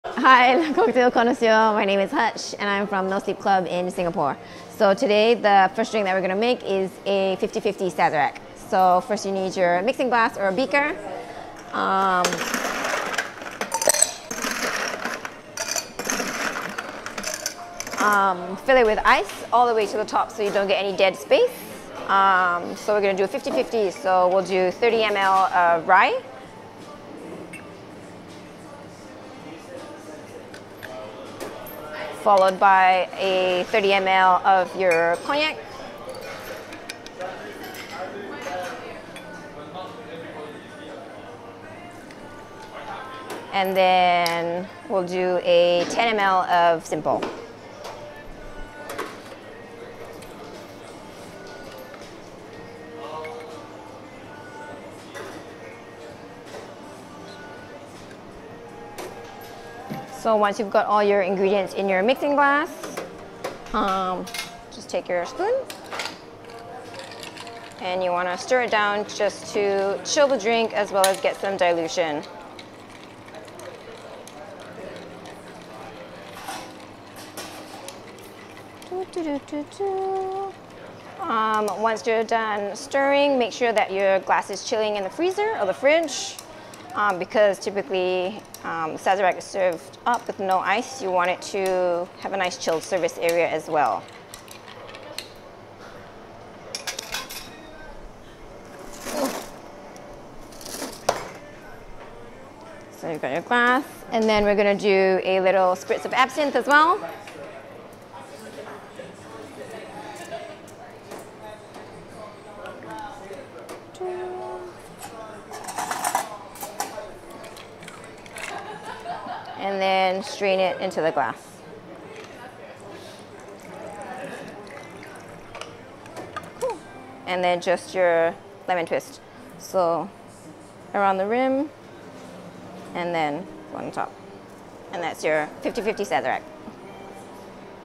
Hutch from No Sleep Club (Singapore): Fifty Fifty Sazerac
Hutch from No Sleep Club makes us her twist on a classic cocktail, Fifty Fifty Sazerac